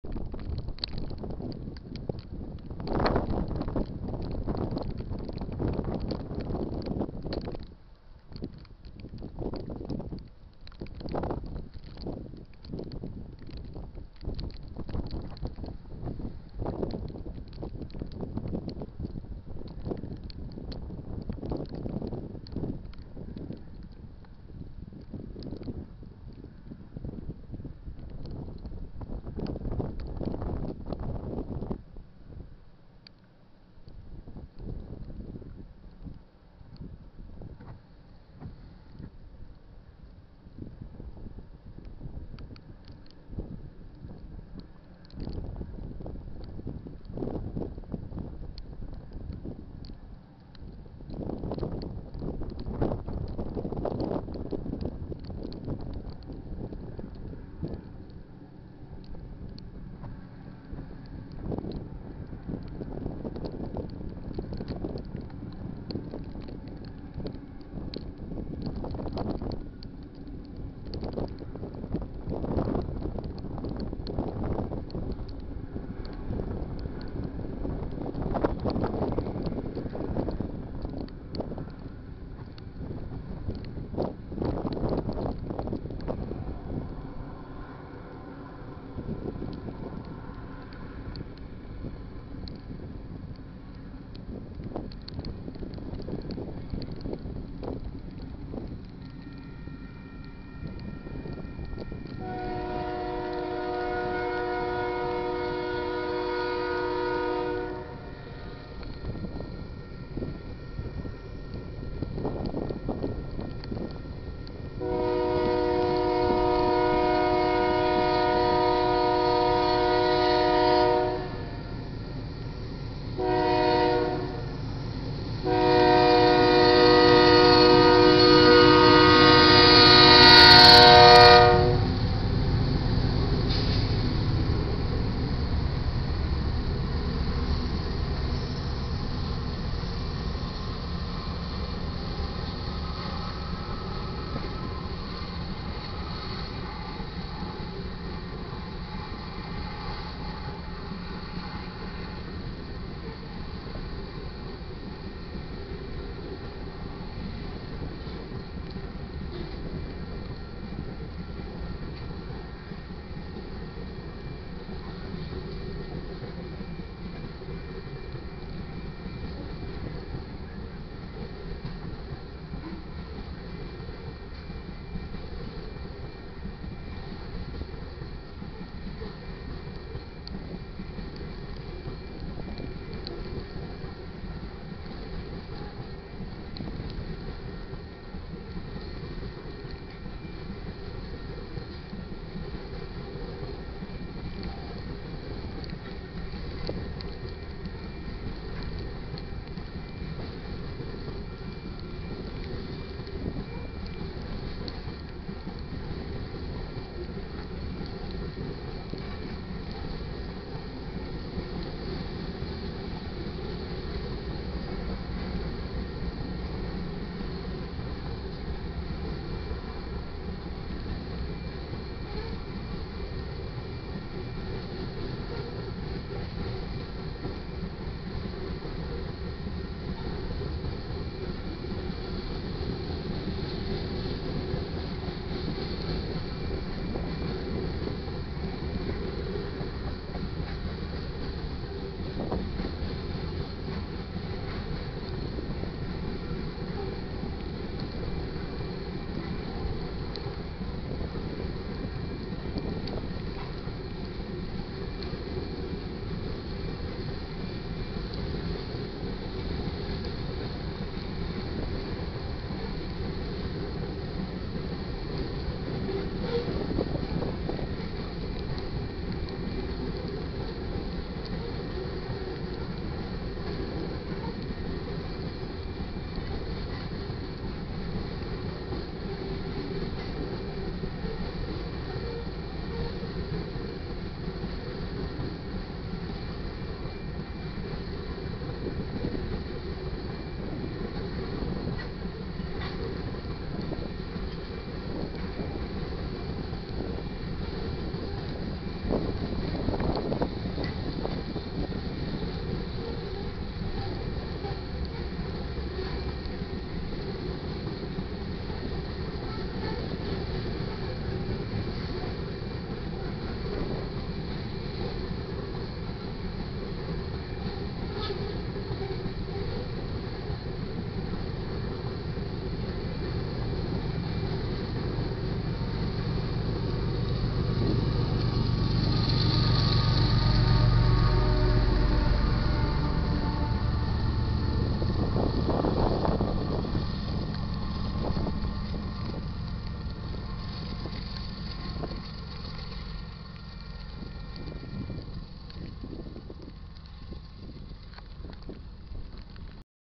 UP 5953 South Fully Loaded Grain Train (4-29-2014)
UP 7683 (GE ES44AC)
South Loaded Grain Train/Main Track/N. Kasper/M. P. 296.35
UP Albert Lea Subdivision/6:48 PM